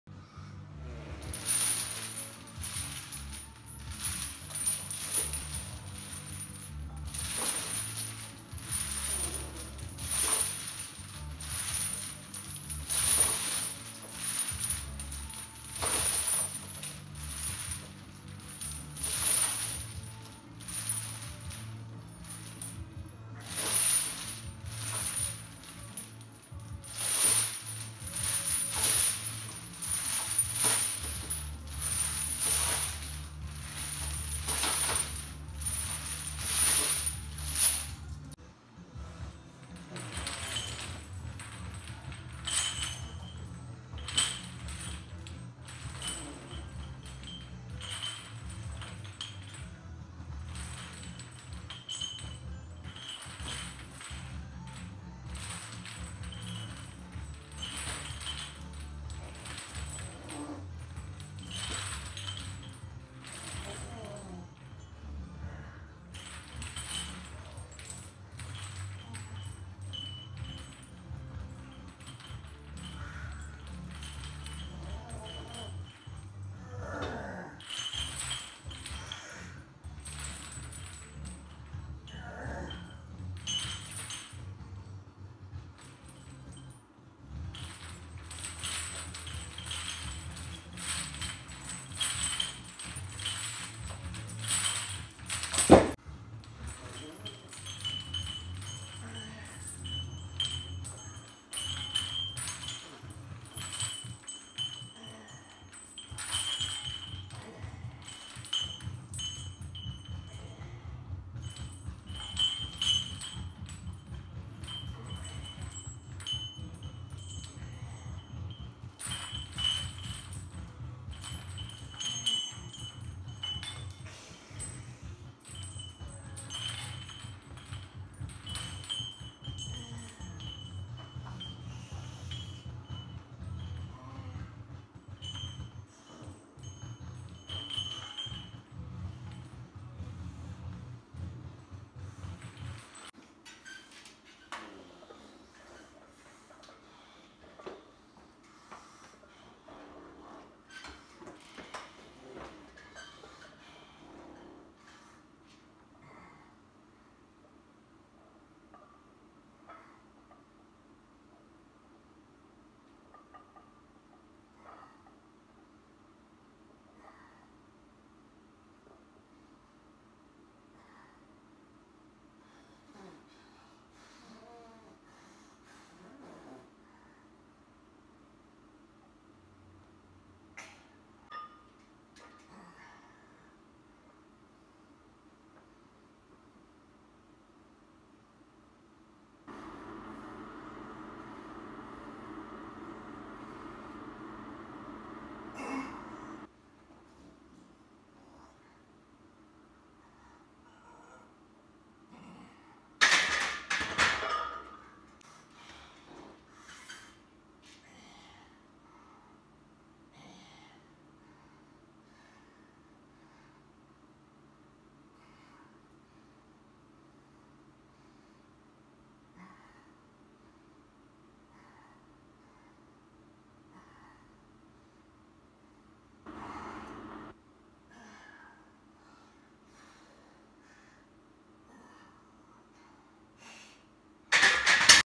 December 11, 2019
10 lb w 67 lb chain x a few. 20 w 50-60 chain x a few.  4 sets incline flies not shown.   95x22, 145x12 not shown.   175x9.  200x1 w drop to145. Good day.  Bunch lats. Chins rear delts